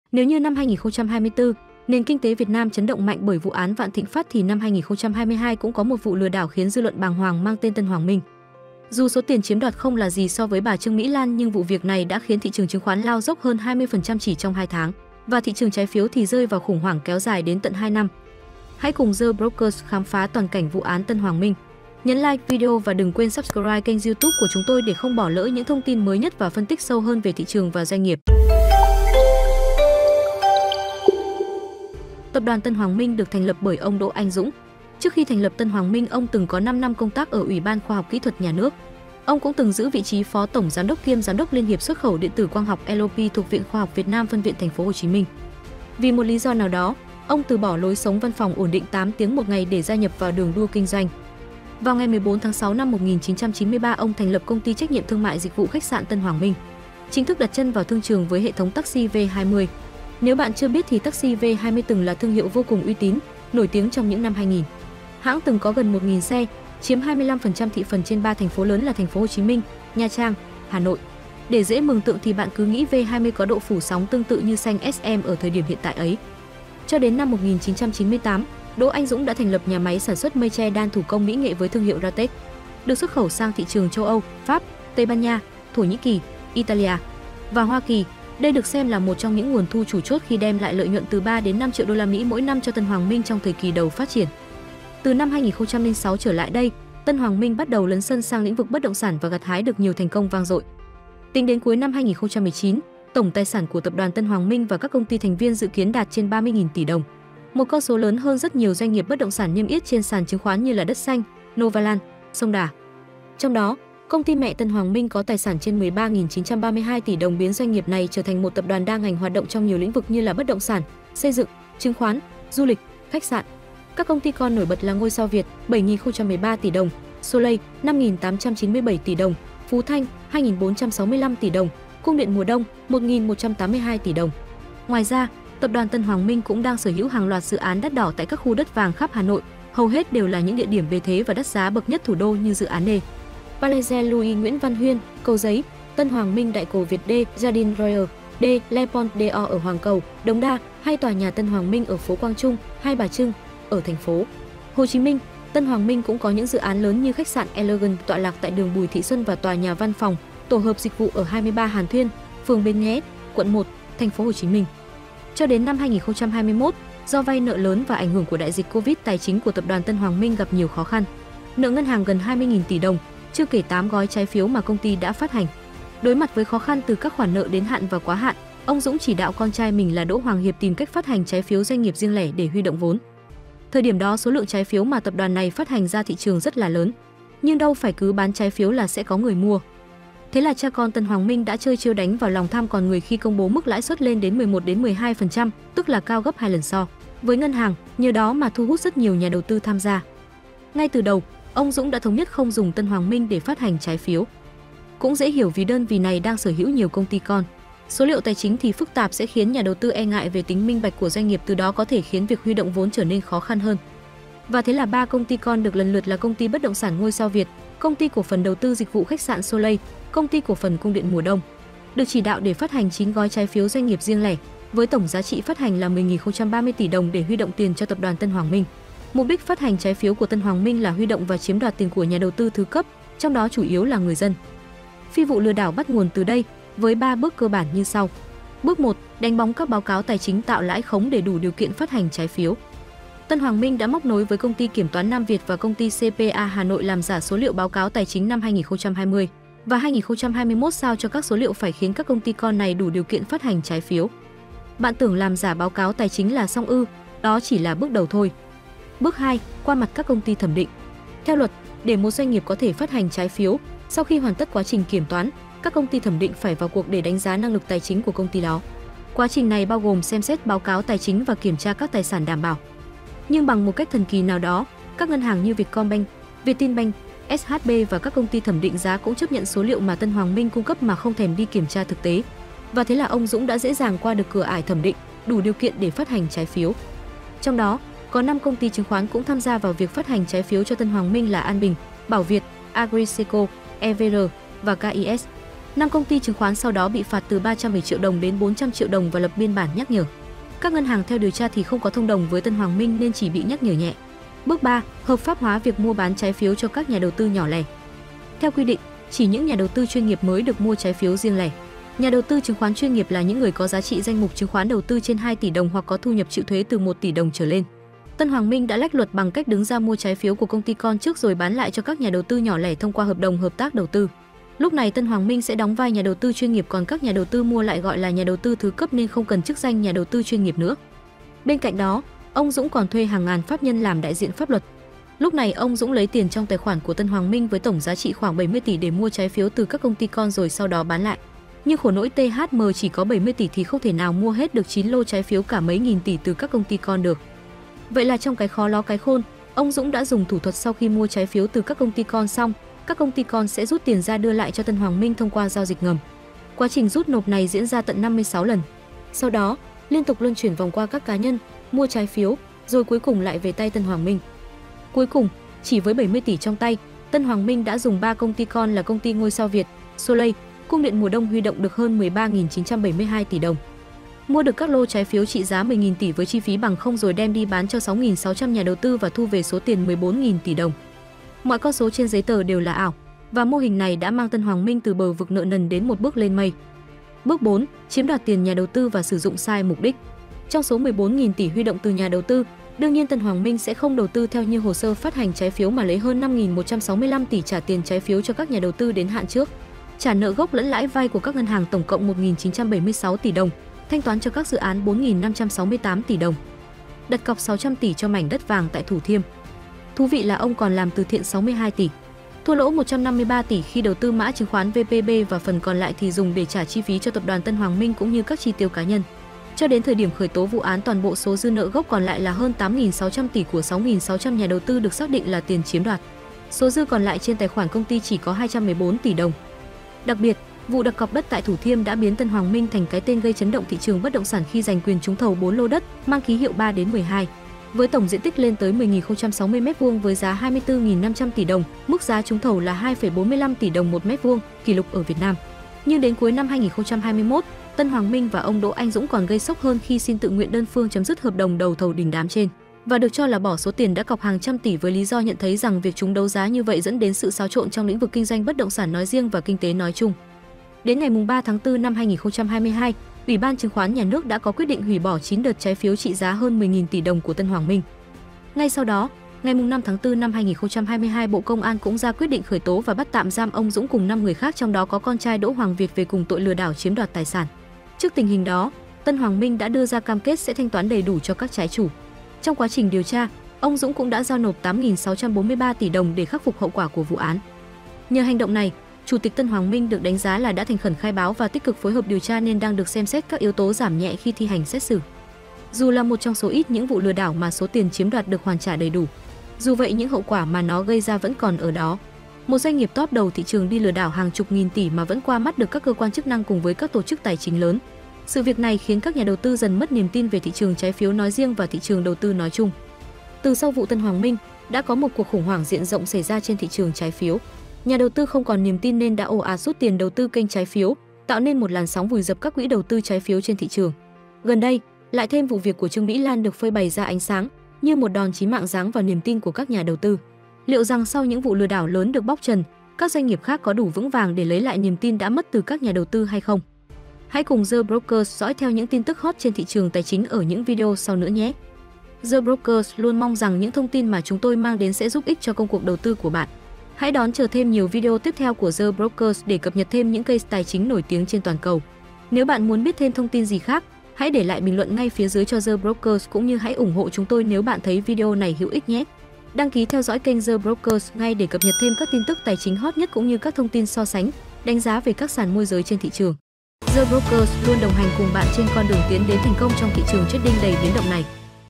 [CASE TÀI CHÍNH] TÂN HOÀNG MINH ĐÃ CHIẾM ĐOẠT 8600 TỶ LÀM "CỦA RIÊNG" NHƯ THẾ NÀO?
Nếu như năm 2024, nền kinh tế Việt Nam chấn động mạnh bởi vụ án Vạn Thịnh Phát thì năm 2022 cũng có một vụ lừa đảo khiến dư luận bàng hoàng mang tên Tân Hoàng Minh.

Dù số tiền chiếm đoạt không là gì so với bà Trương Mỹ Lan nhưng vụ việc này đã khiến thị trường chứng khoán lao dốc hơn 20% chỉ trong 2 tháng và thị trường trái phiếu thì rơi vào khủng hoảng kéo dài đến tận 2 năm.
0:00 Intro
0:35 Giới thiệu về tập đoàn Tân Hoàng Minh
2:50 Thủ đoạn của Tân Hoàng Minh
7:40 Tân Hoàng Minh sụp đổ

🔵 The Brokers - Tra cứu, so sánh và đánh giá sàn giao dịch uy tín
▪ Top sàn Forex uy tín nhất